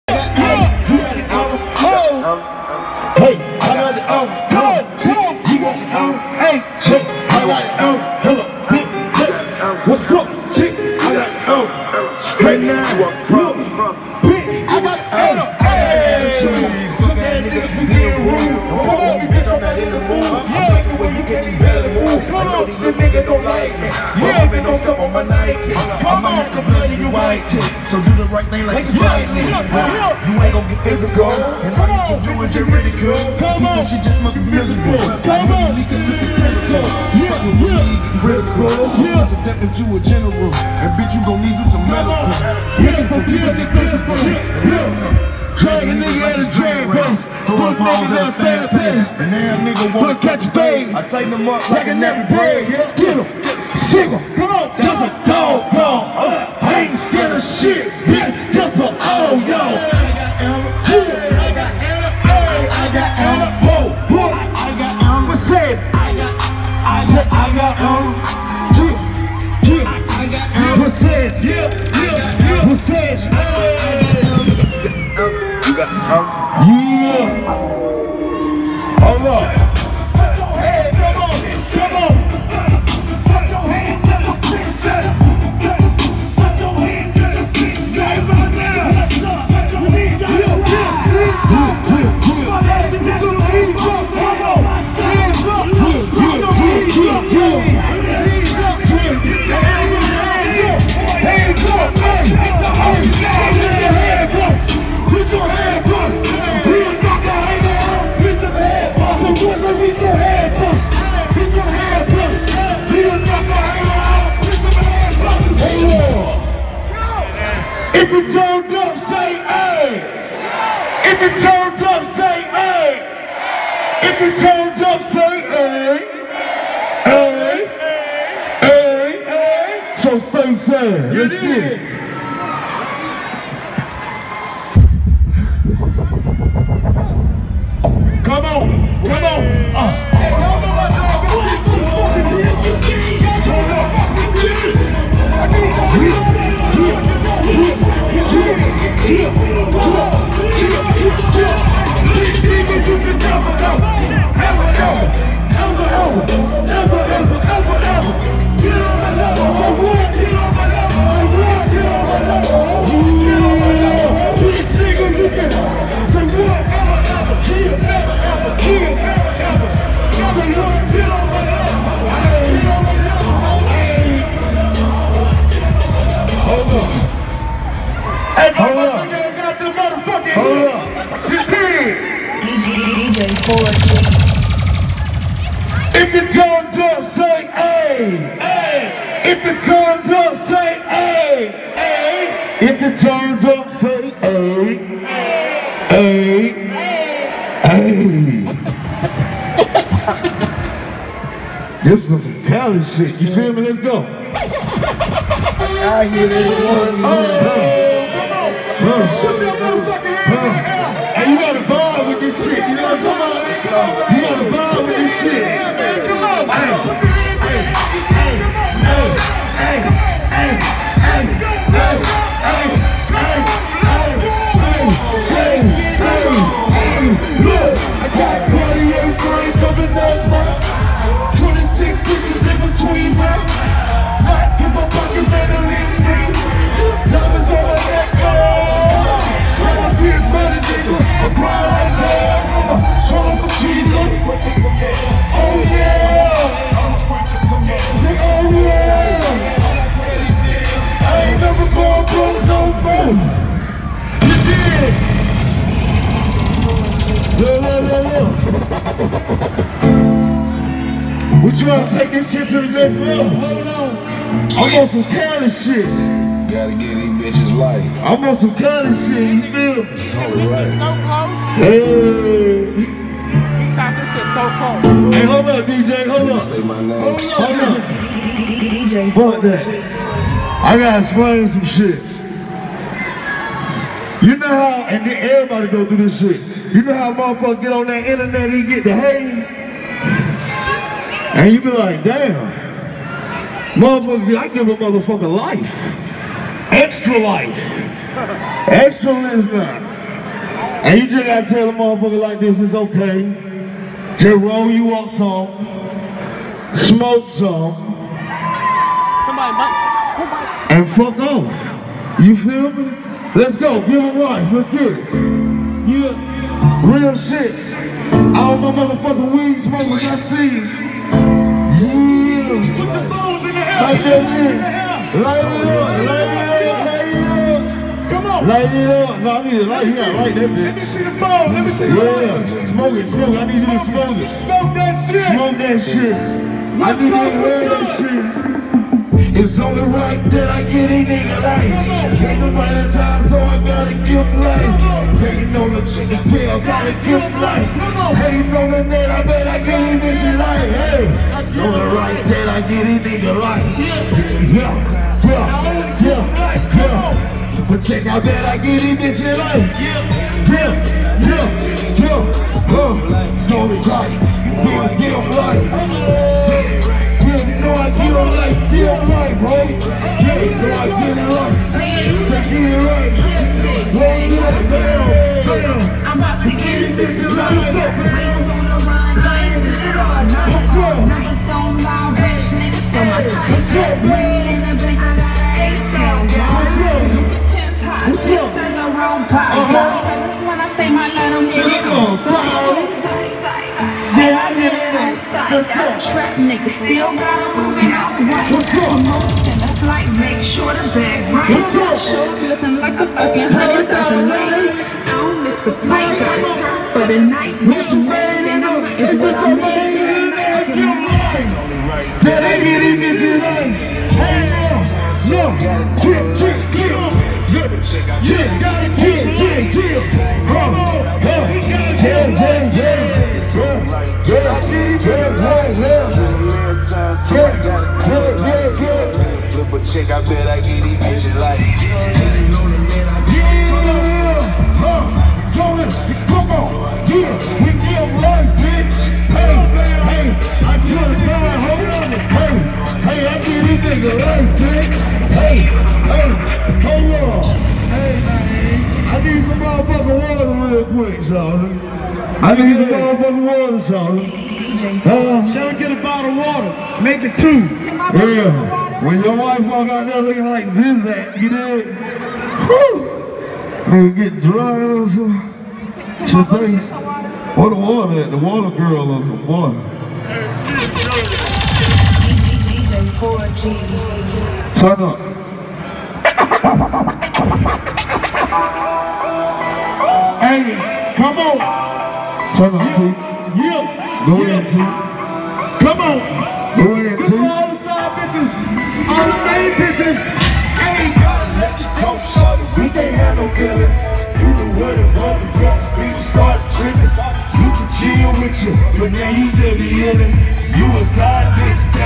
DJ Forgi live at The Citizens Buisness Bank Arena
Me djing lIve for Lil Scrappy at 2chainz concert